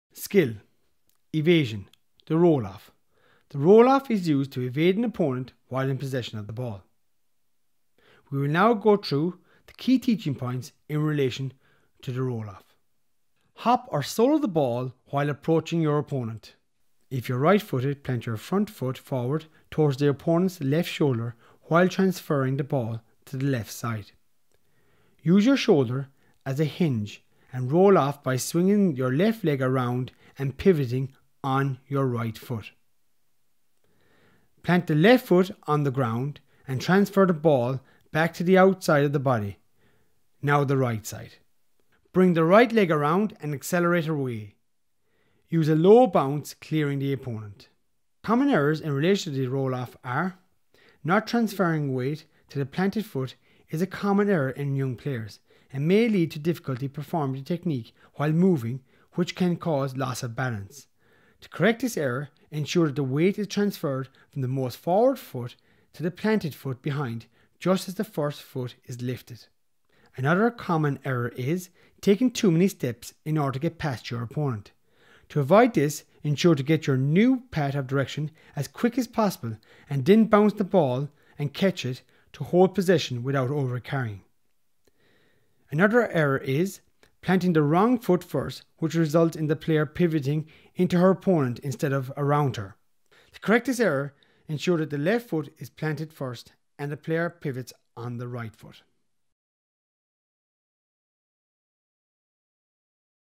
Ladies Gaelic Football Skills - The Roll Off
Players need to use evasion skills throughout ladies gaelic football games. One of these skills on the Roll Off and the key coaching points are described here.